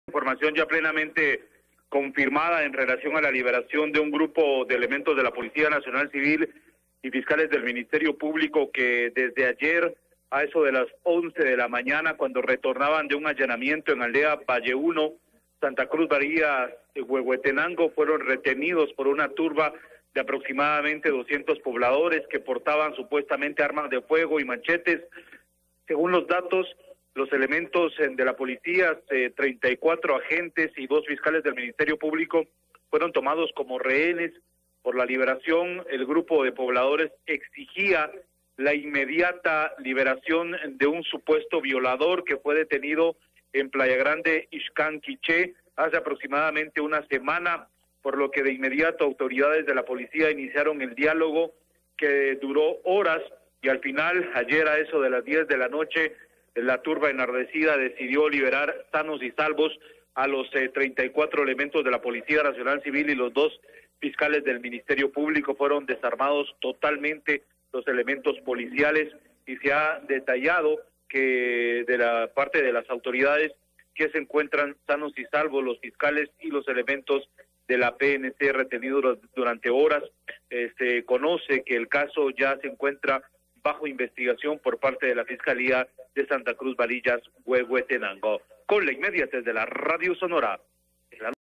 Ministerio de Trabajo realiza Feria de Empleo para personas de 30 a 59 años
Durante esta semana todas las personas entre 30 a 59 años, podrán participar en la Feria de Empleo que promueve el Ministerio de Trabajo y Previsión Social.